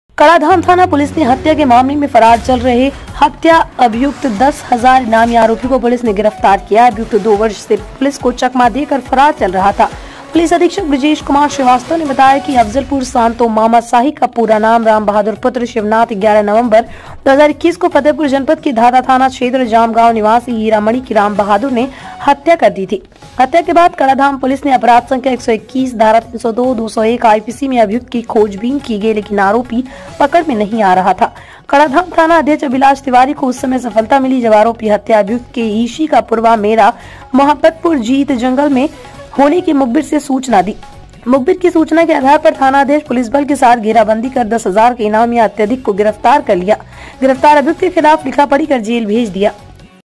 दस हजार के इनानिया हत्या अभियुक्त को पुलिस ने किया गिरफ्तार...
हमसे जुडने के लिए इन लिंक्स पर फॉलो करें:
News Paper
कौशाम्बी कड़ा धाम थाना पुलिस ने हत्या के मामले में फरार चल रहे हत्या अभियुक्त 10,000 इनामियां आरोपी को पुलिस ने गिरफ्तार किया है। अभियुक्त 2 वर्ष से पुलिस को चकमा देकर फरार चल रहा था। पुलिस अधीक्षक बृजेश कुमार श्रीवास्तव ने बताया कि अफजलपुर सांतो मामा साही का पूरा राम बहादुर पुत्र शिवनाथ 11 नवंबर 2021 को फतेहपुर जनपद के धाता थाना क्षेत्र जाम गांव निवासी हीरामणि की राम बहादुर ने हत्या कर दिया था। हत्या के बाद कड़ा धाम पुलिस ने अपराध संख्या 121 धारा 302 201 आईपीसी में अभियुक्त की खोजबीन की गई लेकिन आरोपी पकड़ में नहीं आ रहा था। कड़ा धाम थाना अध्यक्ष अभिलाष तिवारी को उस समय सफलता मिली जब आरोपी हत्या अभियुक्त के ईशी का पुरवा मेरा मोहब्बदपुर जीत जंगल में होने की मुखबिर ने सूचना दिया। मुखबिर की सूचना के आधार पर थाना अध्यक्ष पुलिस बल के साथ घेराबंदी कर 10000 के इनानिया अत्याधिक को गिरफ्तार कर लिया गिरफ्तार अभियुक्त के खिलाफ लिखा पढ़ी कर जेल भेज दिया।